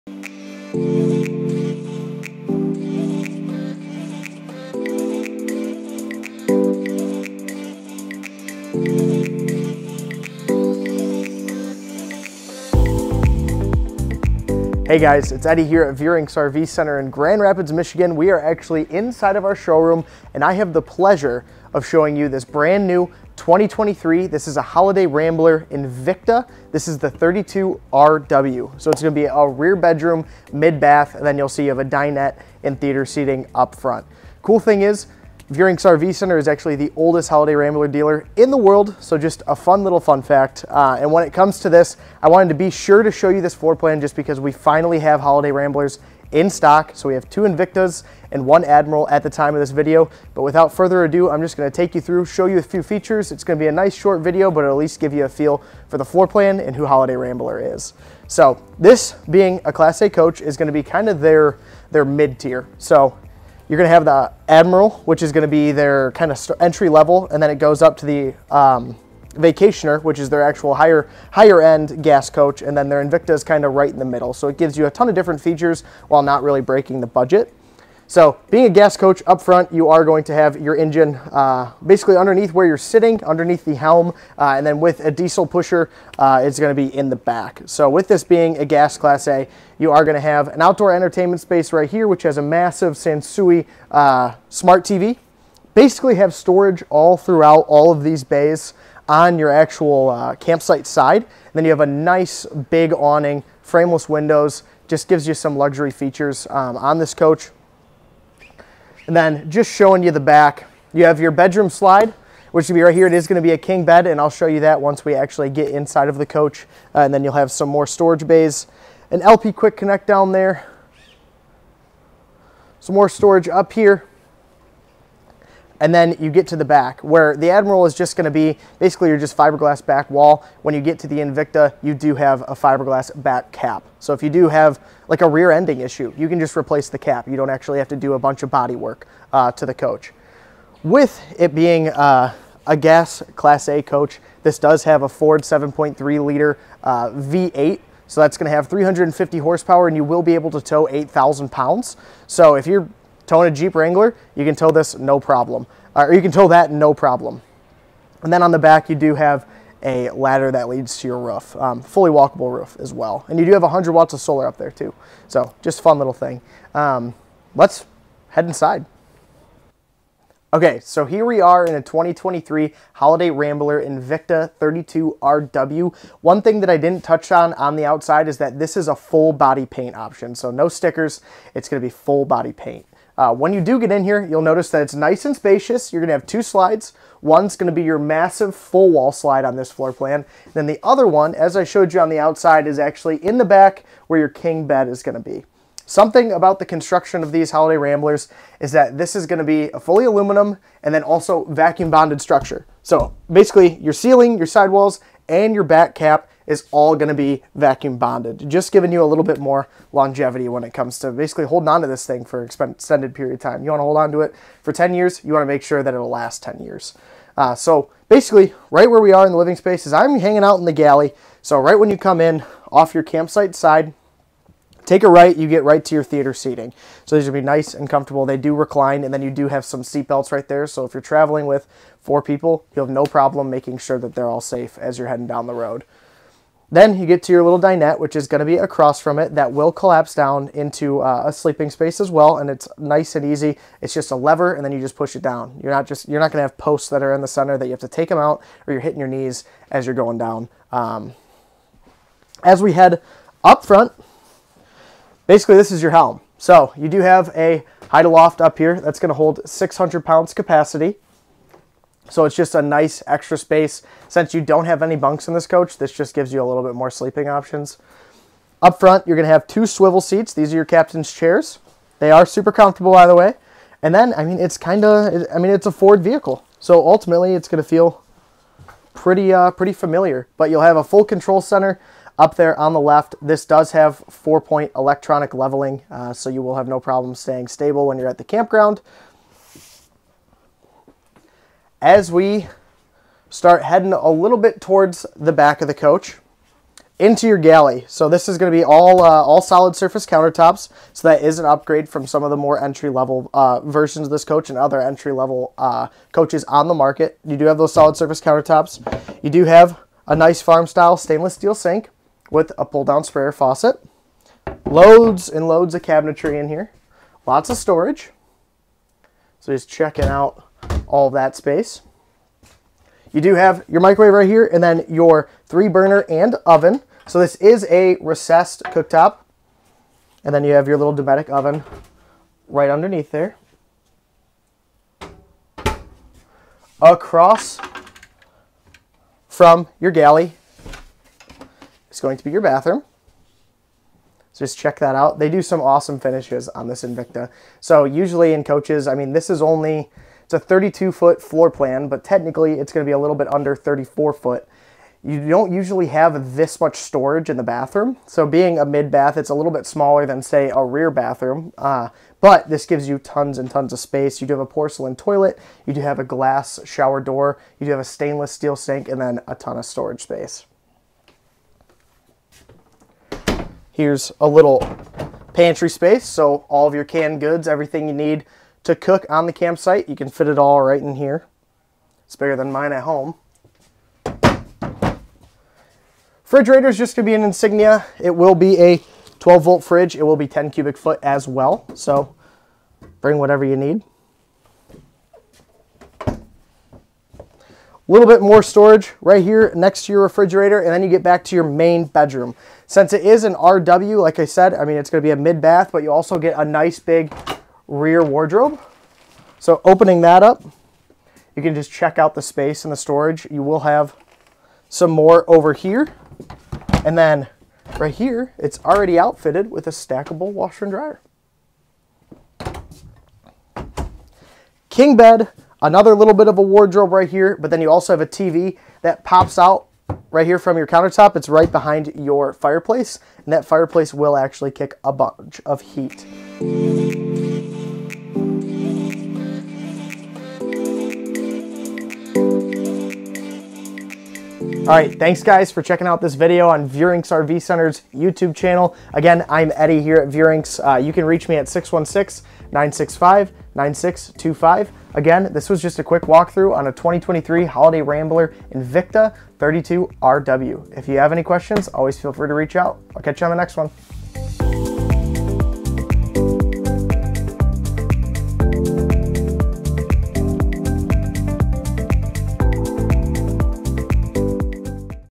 2023 New Holiday Rambler Invicta 32RW Class A Gas For Sale | Features Tour | RV Dealer in MI, IN, OH
Eddie is showcasing this all new Holiday Rambler Invicta 32RW! Check out the beautiful exterior color and step inside to see the fresh interiors and massive wardrobe in this one! This is just a glimpse of the Holiday Rambler Invicta - if you have any questions or want to make this yours...reach out to Eddie!

GRAND RAPIDS LOCATION:
7144 Division Ave. S.